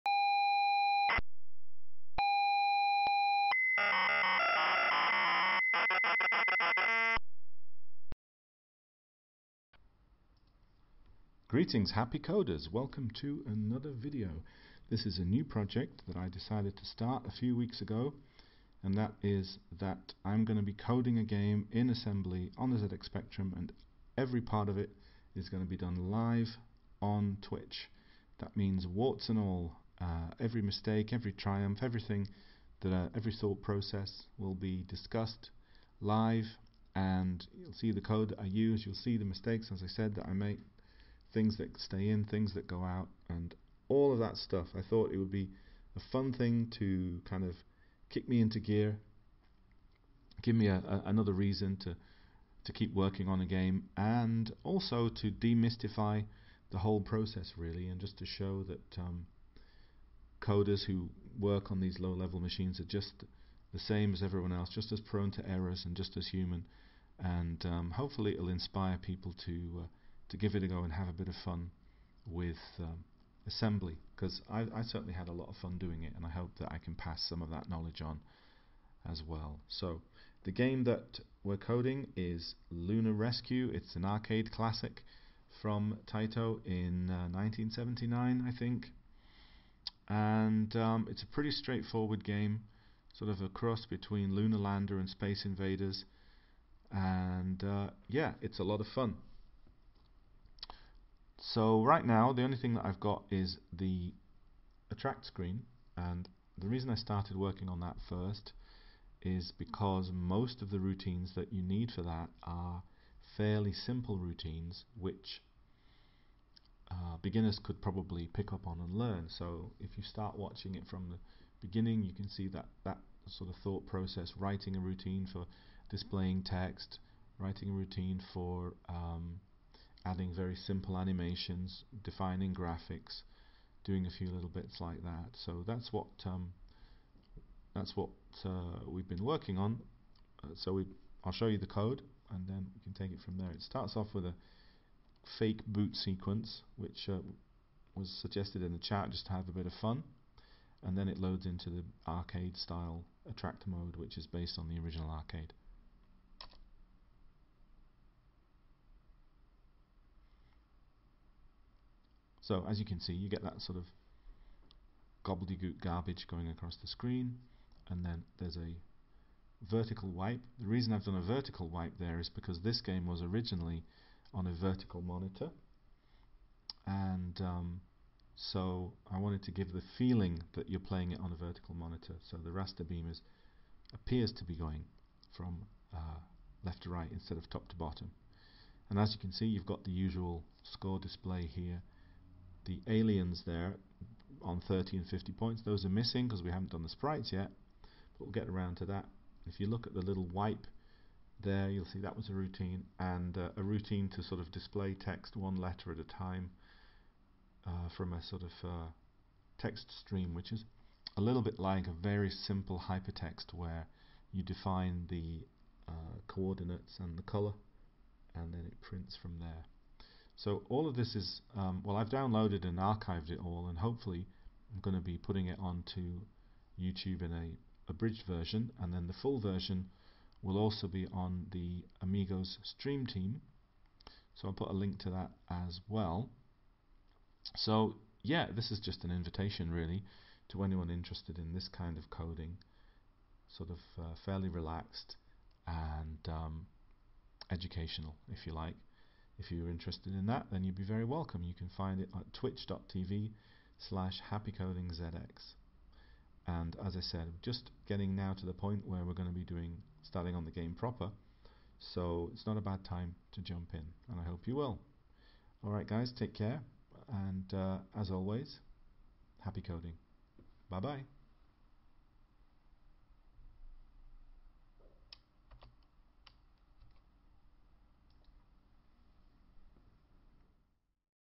Live Coding Lunar Rescue
I've started a new project in which I code an entire game live in assembly on the ZX Spectrum live on Twitch with nothing cut out. Warts and all, every tragedy and triumph from start to finish. Hope you'll drop in sometime.

older streams will be removed from Twitch but archived by the Amigos Stream Team.

Hopefully an edited version of the streams will also make its way to Youtube eventually.